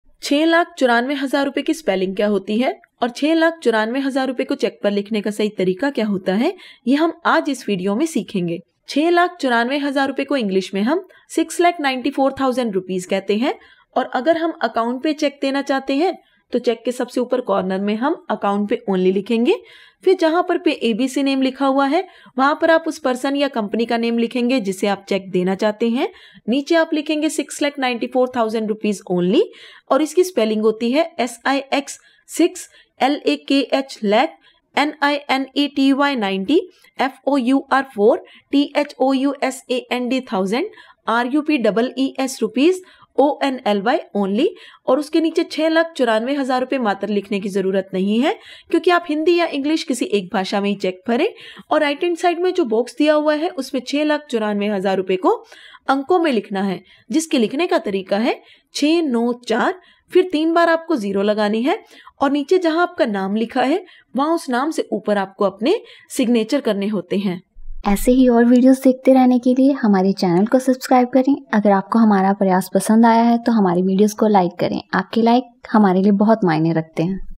694000 ko english mein kya kahate hain | 694000 in words | 694000 ki English | 694000 spelling
694000 ko english mein kya kahate hain | 694000 in words | 694000 ki English | 694000 spelling

तो बस एक मिनट में सीखिए 694000 को cheque par kaise likhe
Let’s learn how to write 694000 in English.

694000 का अंग्रेजी में अर्थ होता है
694000 रुपये को Cheque पर लिखने का सही तरीका :

Six lakh ninety four thousand Rupees only
छः लाख चौरानवे हज़ार रुपये मात्र

694000/-

account payee